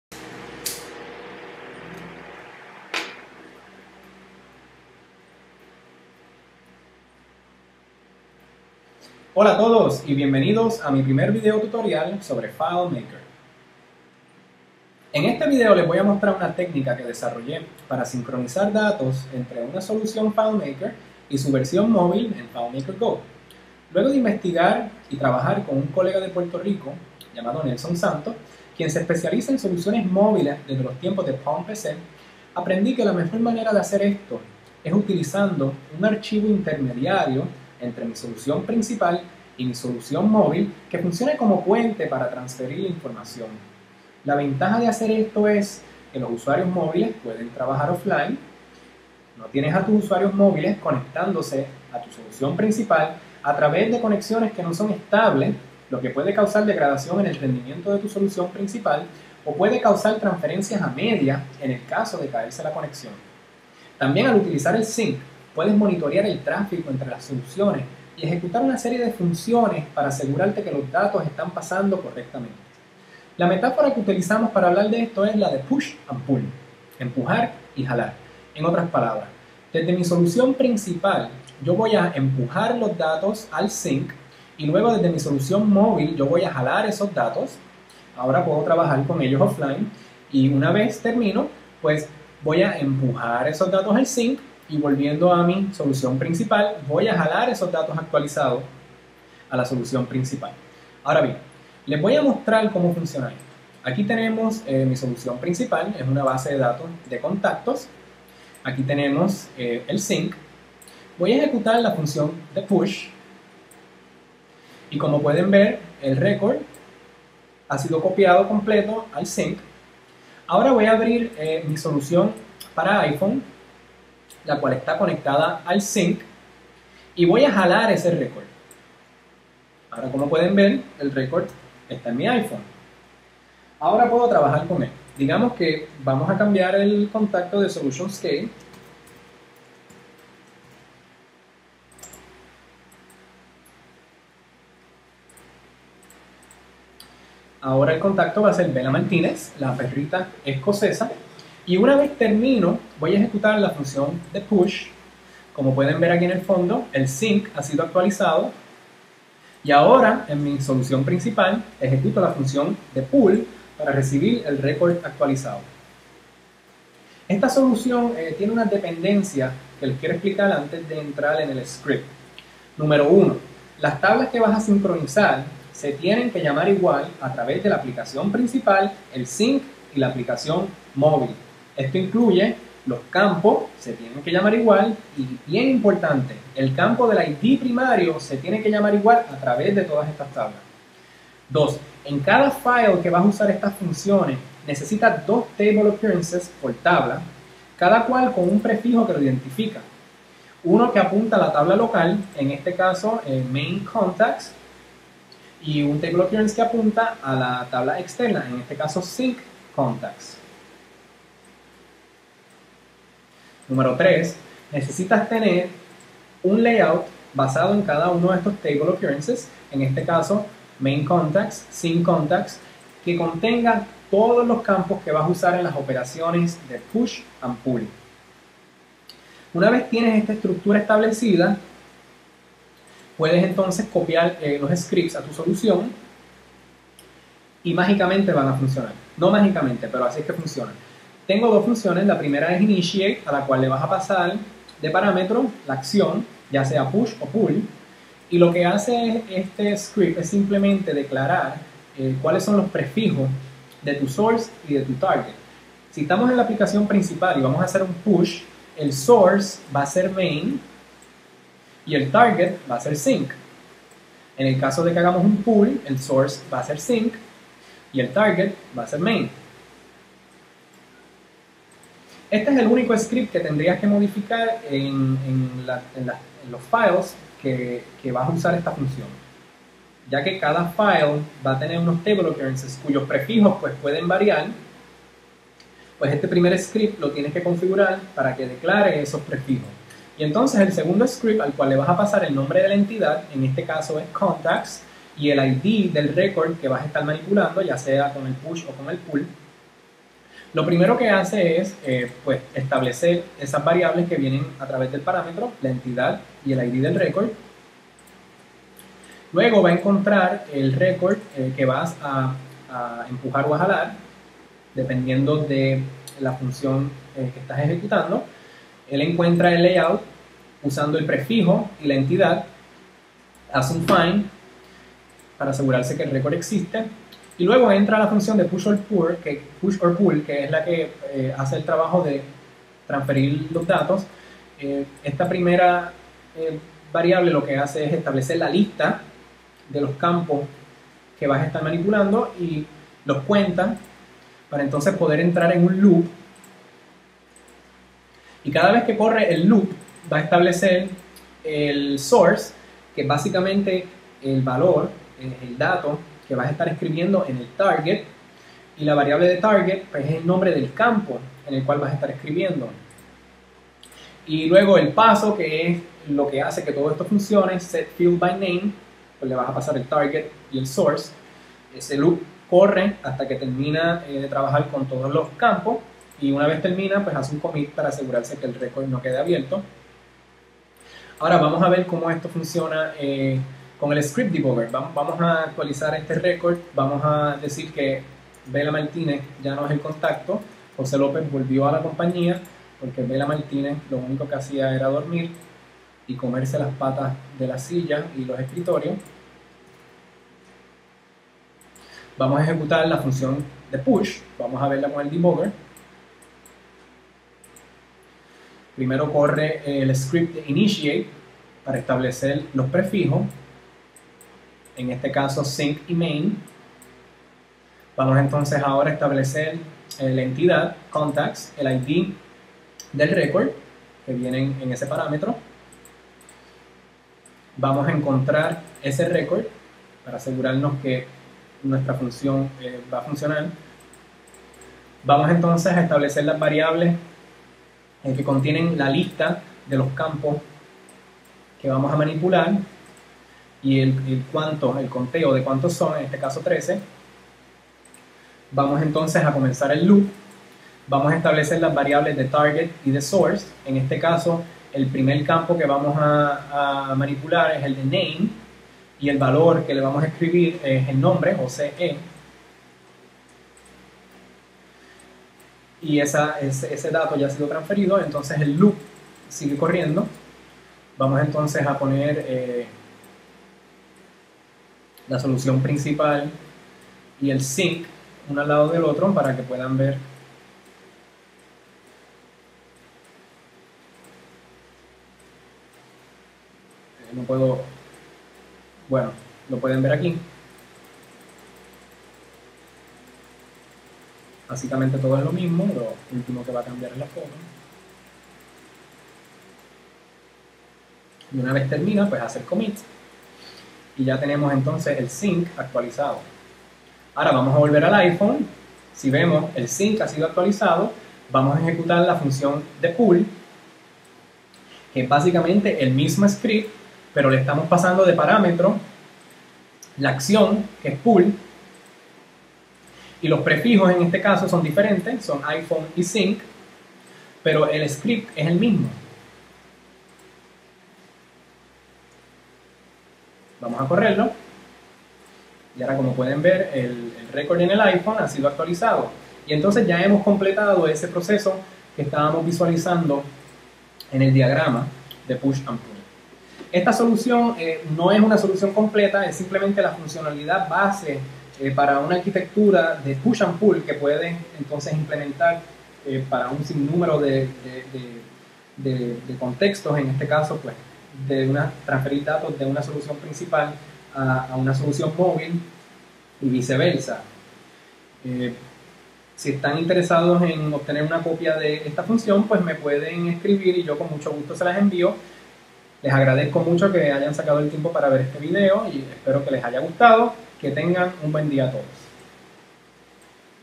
Técnica para sincronizar datos entre FileMaker y FMGo
En este video muestro como sincronizar datos entre una solución principal en FileMaker y su versión móvil en FMGo con un solo script y un archivo intermediario.

In this video I show a technique for synchronizing data between a main FileMaker solution and its mobile FMGo version using one script and an intermediary Sync file.